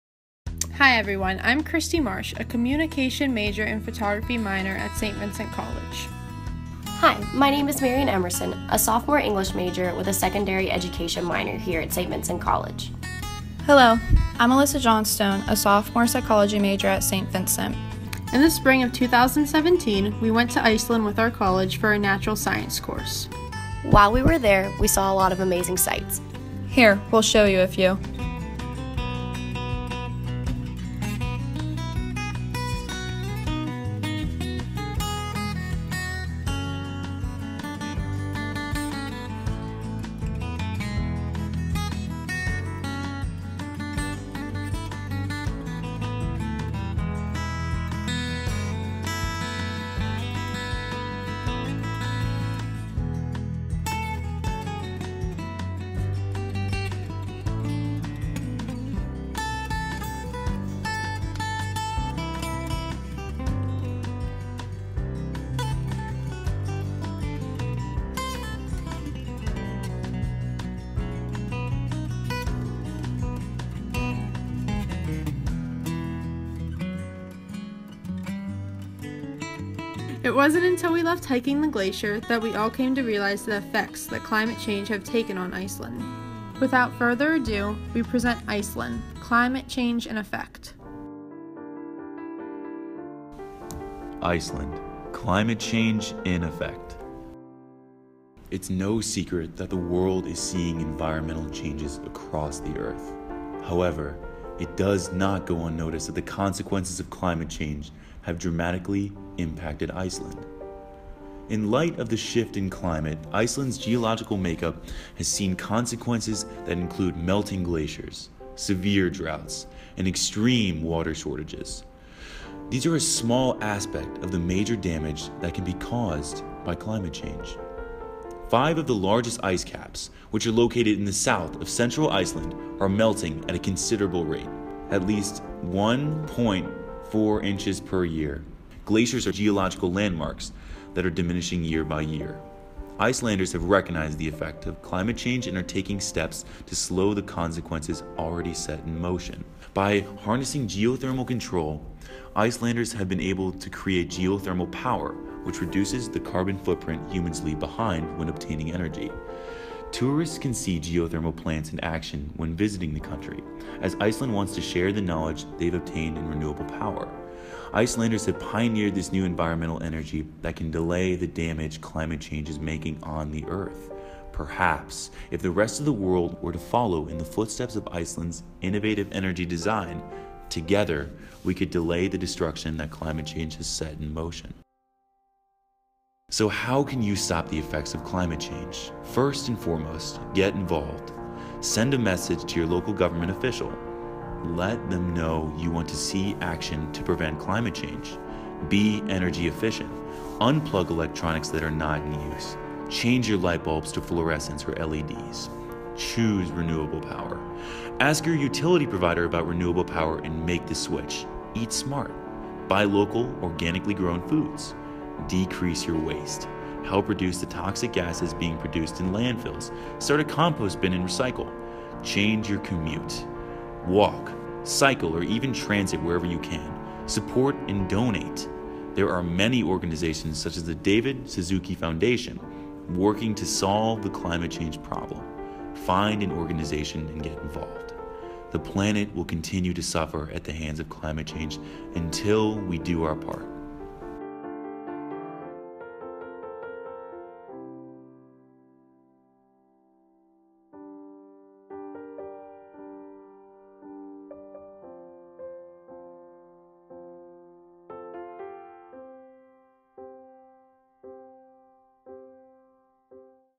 Iceland: Climate Change in Effect
| Group video project presented by Cristy Marsh, Marion Emerson, and Alyssa Johnstone at the 14th annual Academic Conference. Iceland is a prime example of how the difficulties of climate change are affecting our world.